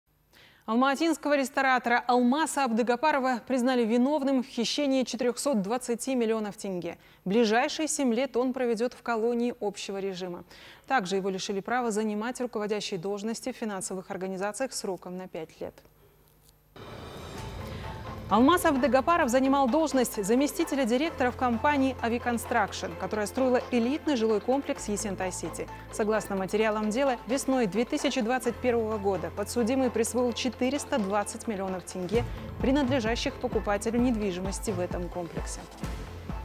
Ресторатора Алмаса Абдыгаппарова признали виновным в хищении 420 миллионов тенге
Алматинского ресторатора Алмаса Абдыгаппарова признали виновным в хищении 420 миллионов тенге. Ближайшие 7 лет он проведет в колонии общего режима. Также его лишили права занимать руководящие должности в финансовых организациях сроком на пять лет.
Алмас Абдыгапаров занимал должность заместителя директора в компании "Ави Констрашн", которая строила элитный жилой комплекс Esentai City. Согласно материалам дела, весной 2021-го года подсудимый присвоил 420-ть миллионов тенге, принадлежащих покупателю недвижимости в этом комплексе.